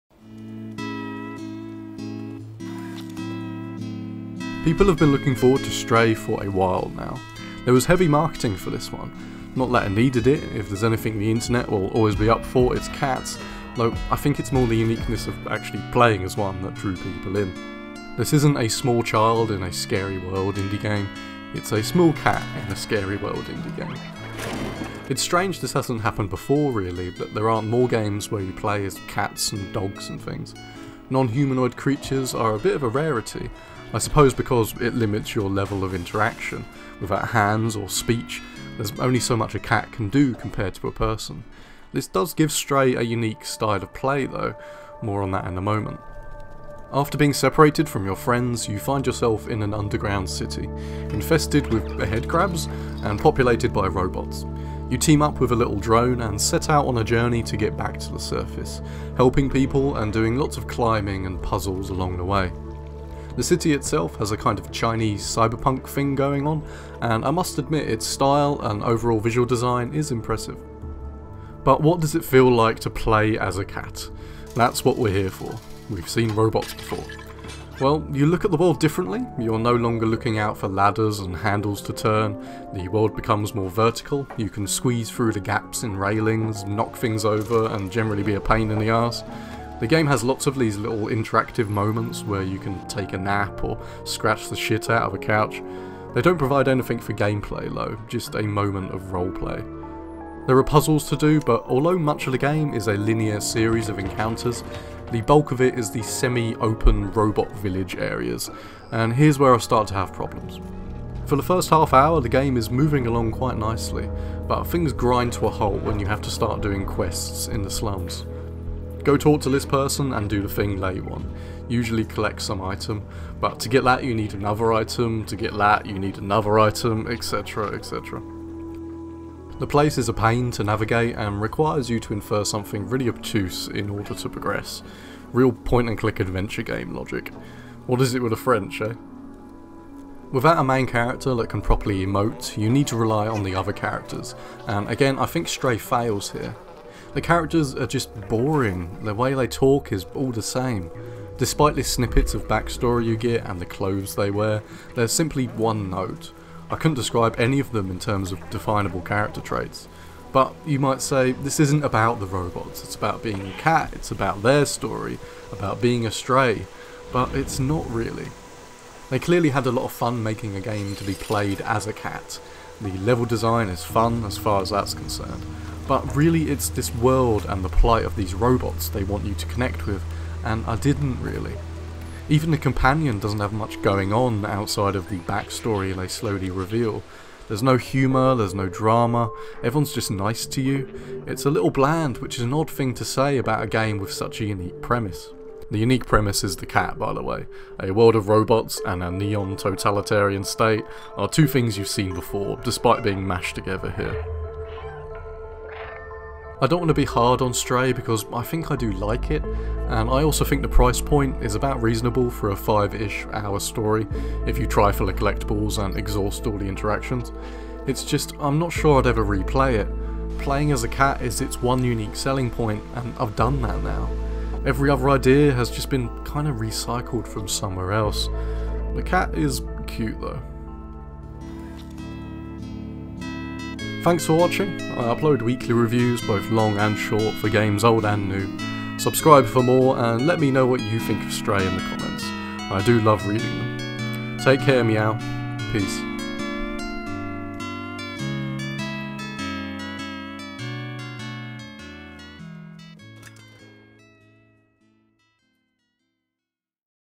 Stray Game Review
This isn't a small child in a scary world indie game, its a small cat in a scary world indie game. This French indie game has garnered a lot of press, but is it deserved?